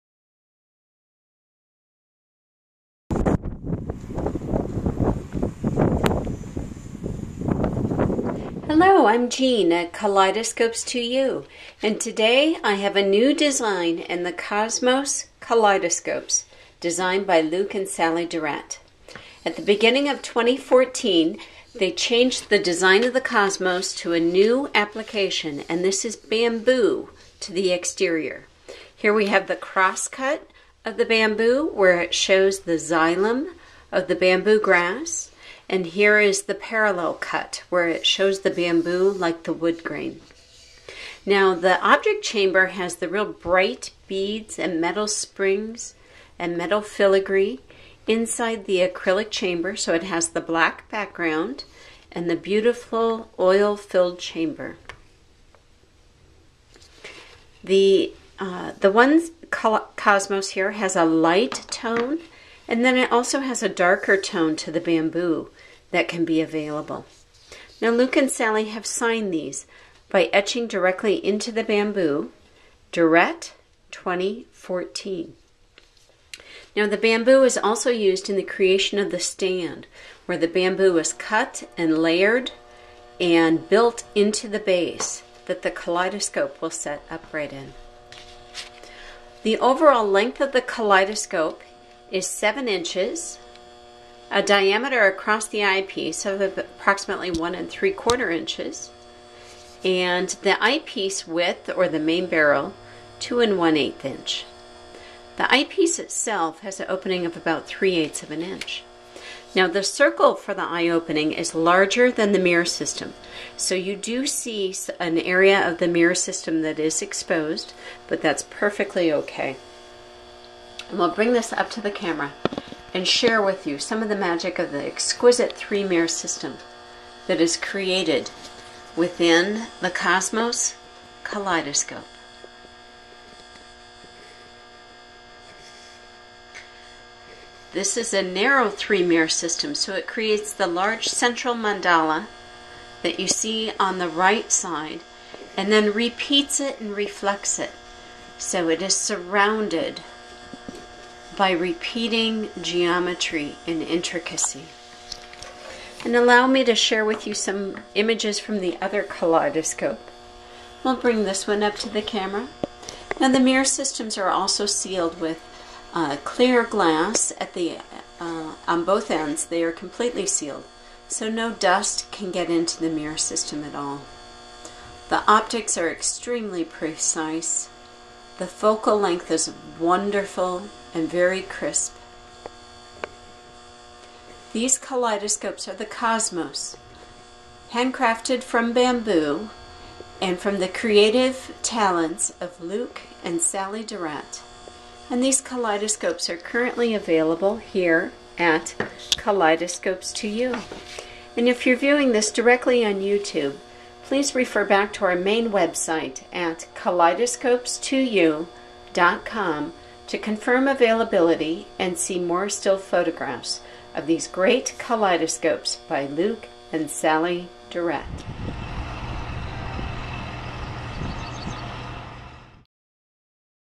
New Cosmos by Luc and Sallie Durette in Sustainable Bamboo
Beautiful new handheld kaleidoscopes in 2014